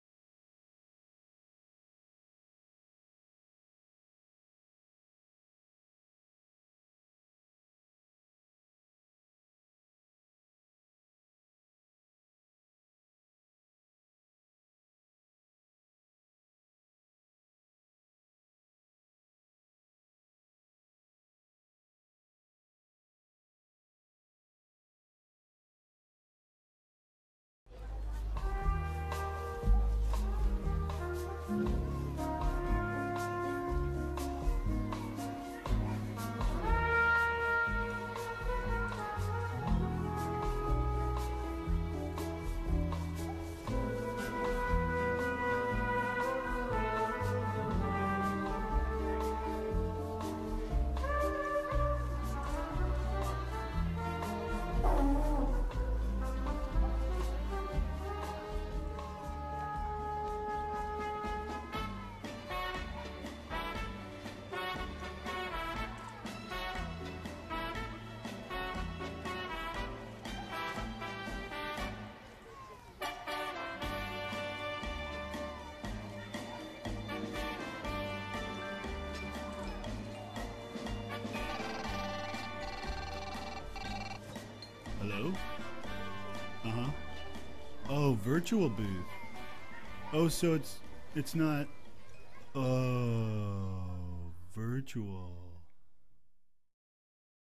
Emma | Virtual Conference Booth Video
We were recently a vendor at a "virtual" conference hosted by MarketingProfs. The virtual booth software included the option to post a short video clip to welcome visitors. But what's the best way to say hello to virtual visitors in a virtual booth? By reminding them of just how classy *real* conference booths can be, of course. Here's our video...